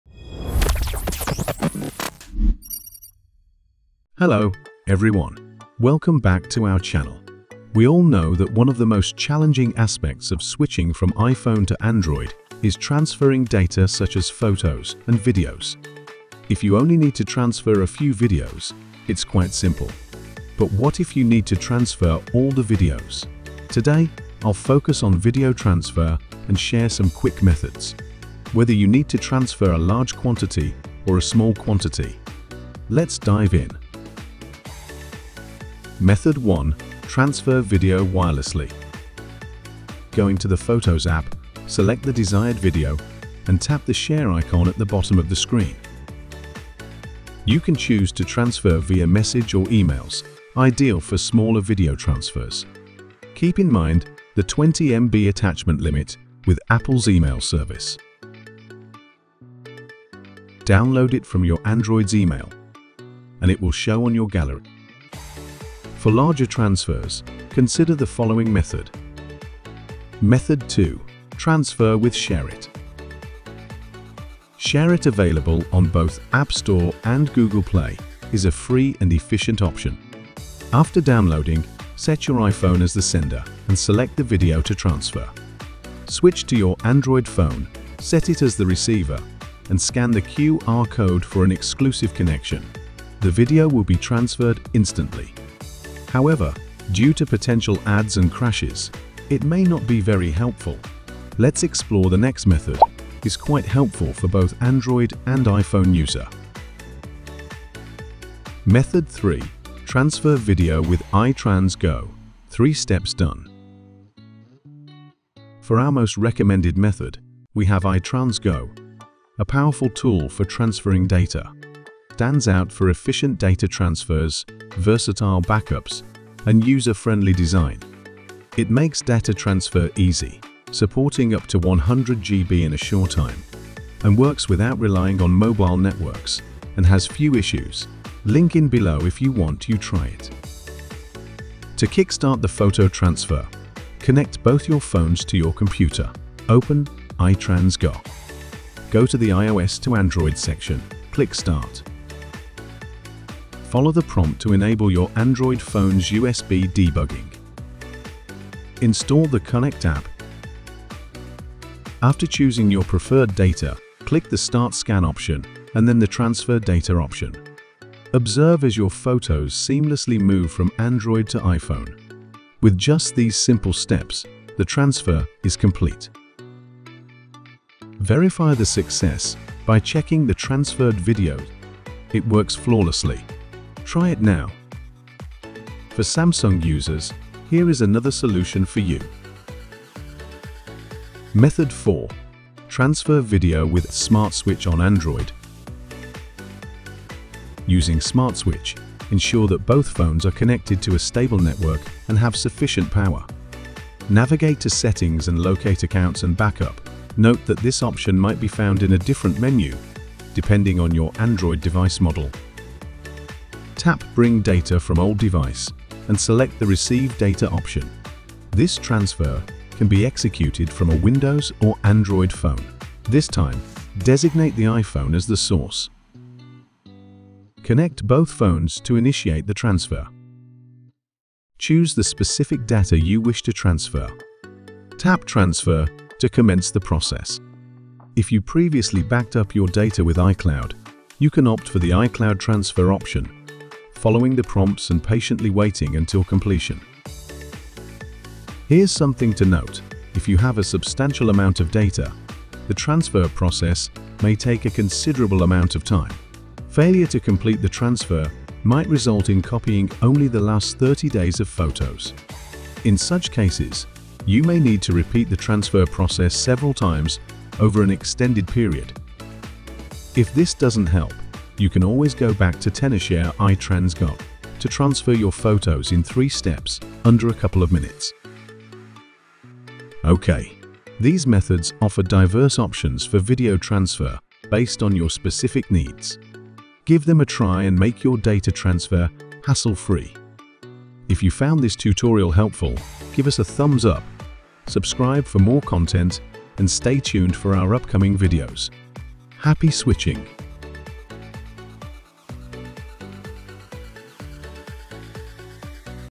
[2024 Updated] How to Transfer Videos from iPhone to Android 2024| Full-Quality | 4 Ways
Need to switch from iPhone to Android and face the challenge of transferring photos and videos? Whether it's a handful or an entire collection, worry not! Today, I've got you covered with quick and effective video transfer methods.

**Method 1: Wireless Video Transfer**
- Open Photos on iPhone.
- Select video and share via message or email for smaller transfers.
- Download from Android's email.
v
**Method 2: Transfer with Share It**
- Download ShareIt.
- Set iPhone as sender, Android as receiver, transfer via QR code.
- Fast but watch out for ads and crashes.

**Method 3: Transfer Video with iTransGo**
- Connect phones to computer.
- Open iTransGo, click start.
- Follow prompts, transfer videos seamlessly.

**Method 4: Transfer Photos with Smart Switch on Android**
- Ensure stable network and power.
- Go to settings TAP "Accounts and Backup."
- Tap "Bring data from old device"  TAP"Receive data."
- Connect phones, choose data, tap "Transfer."
- Note: For large data, it may take time. Repeat if needed.

If these methods don't suffice, Tenorshare iTransGo offers a quick solution for photo transfer in 3 steps. Try these options for hassle-free data transfer!

TIMESTAMP:
00:00 Introduction
00:35 Method 1: Wireless Video Transfer
01:10 Method 2: Transfer with Share It
01:50 Method 3: Transfer Video with iTransGo – 3 Steps Done
03:10 Method 4: Transfer Photos with Smart Switch on Android (without a computer)